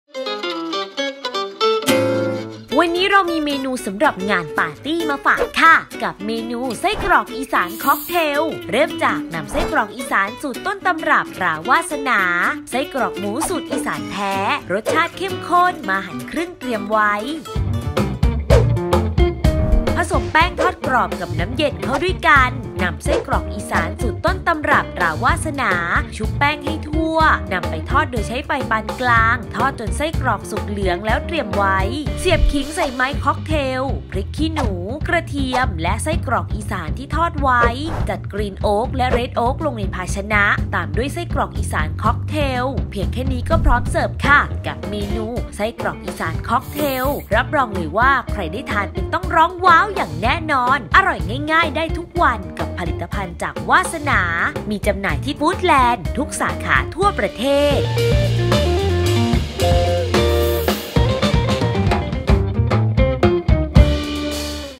ไส้กรอกอีสานค็อกเทล สูตรเด็ด...ทำง่าย...สไตล์ "อ.ยิ่งศักดิ์"
วันนี้อาจารย์มีเมนูสำหรับงานปาร์ตี้มาฝากคร่ะ กับเมนู ไส้กรอกอีสานค็อกเทล จากผลิตภัณฑ์ ตราวาสนา

สิ่งที่ต้องเตรียมสำหรับ(ไส้กรอกอีสานค็อกเทล)

ไส้กรอกอีสาน สูตรต้นตำรับ ตราวาสนา 4 ชิ้น
ไม้ค็อกเทล สำหรับเสียบ
ขิงหั่นเต๋าเล็ก, พริกขี้หนูแดงซอย, กระเทียมหั่นเป็นแว่น, กรีนโอ๊ค, เรดโอ๊ค สำหรับรับประทานคู่

วีธีทำ(ไส้กรอกอีสานค็อกเทล)

1. นำไส้กรอกอีสาน สูตรต้นตำรับ ตราวาสนามาตัดเป็นชิ้นเอาเชือกออก จากนั้นหั่นครึ่ง เตรียมไว้
2. ผสมแป้งทอดกรอบ กับน้ำเย็นเข้าด้วยกันนำไส้กรอกอีสาน สูตรต้นตำรับ ตราวาสนาลงชุบแป้งให้ทั่ว นำไปทอดใช้ไฟปานกลาง ทอดจนไส้กรอกสุกเหลือง ตักขึ้น เตรียมไว้
3. เสียบขิง พริกขี้หนู กระเทียม และไส้กรอกที่ทอดไว้ ด้วยไม้ค็อกเทลให้สวยงาม
4. จัดกรีนโอ๊ค และเรดโอ๊คลงในแก้ว วางไส้กรอกอีสานค็อกเทล พร้อมเสิร์ฟติดตามข่าวสาร ความบันเทิง รวมถึงเทคนิคต่างๆและสูตรลับ เมนูอาหาร ลงโฆษณาสินค้า

▲Line @yingsakfood